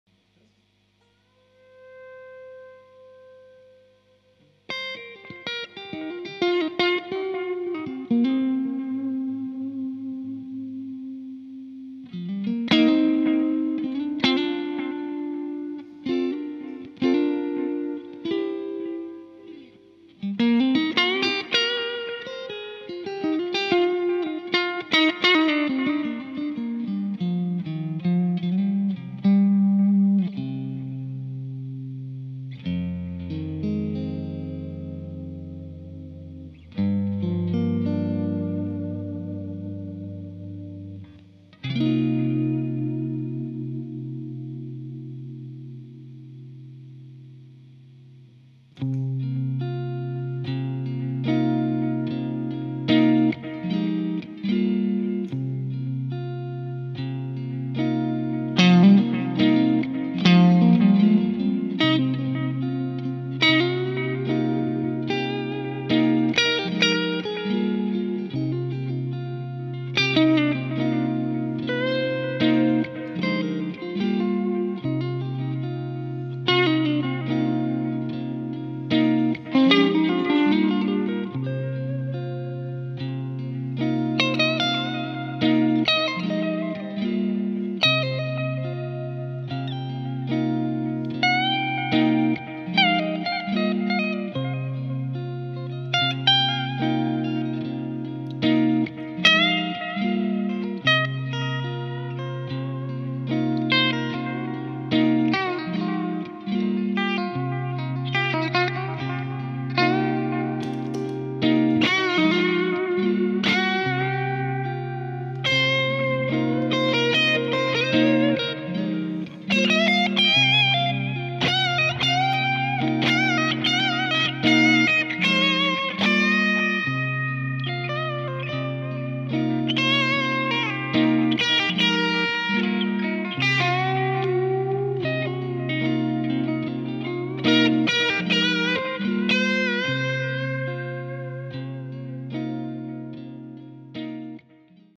1969 Fender Stratocaster
TC Ditto Looper / clean & Okko Diablo "Gregor Hilden Signature Model" / Amp: Fender Vibrolux (original 1966), Vahlbruch Spacetime Delay, TC Polytune Tuner, ChickenPick Picks